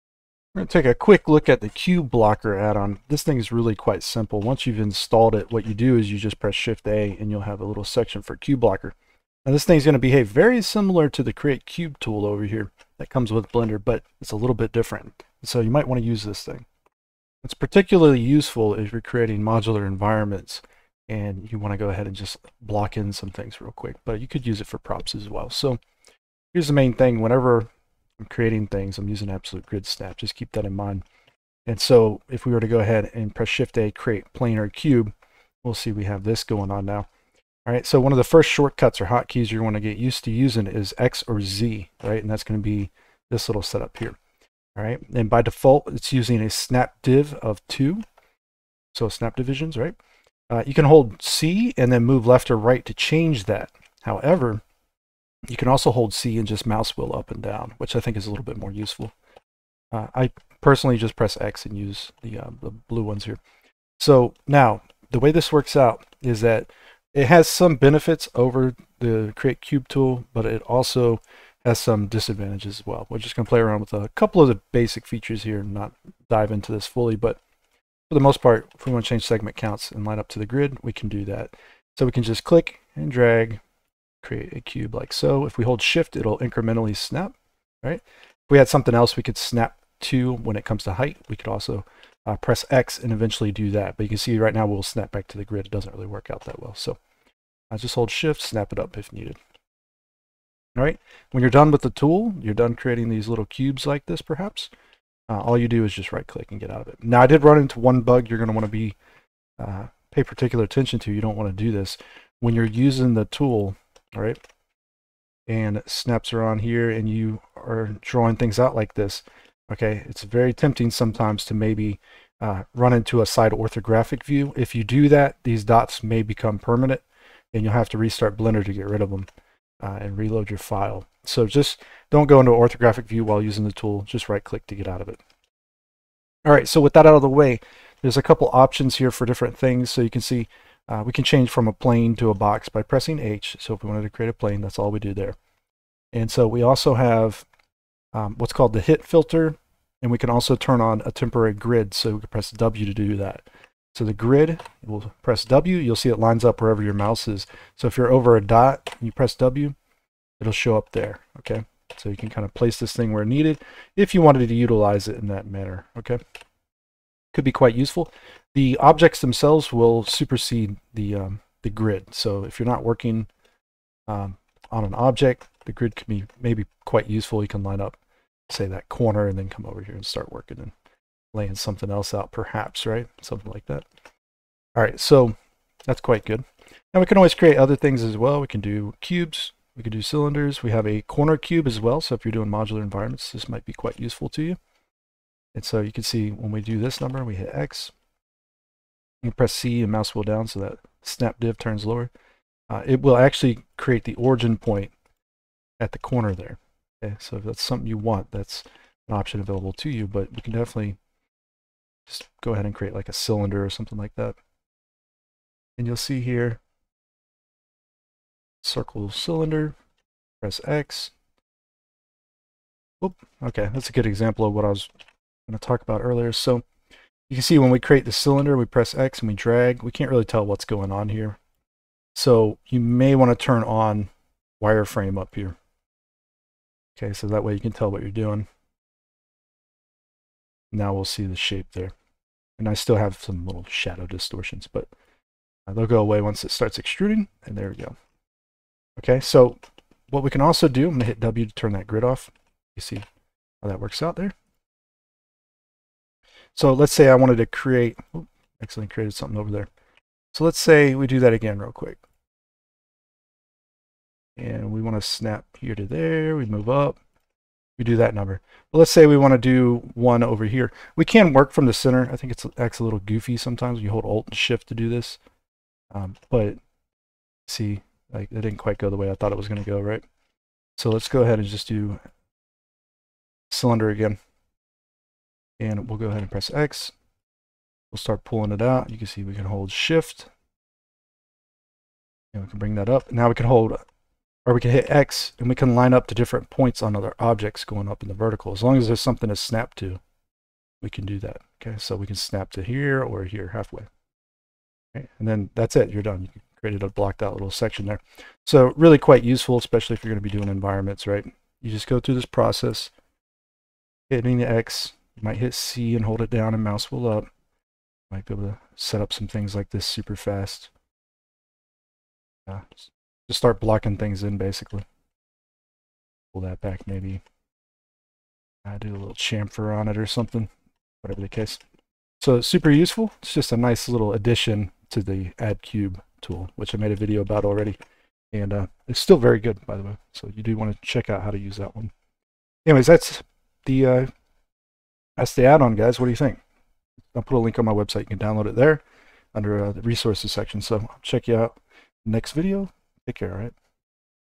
Blender - QBlocker Addon (Free)
Taking a quick look at the QBlocker addon. This addon is very useful for creating block outs / grey boxes. Looking for Blender 3d resources?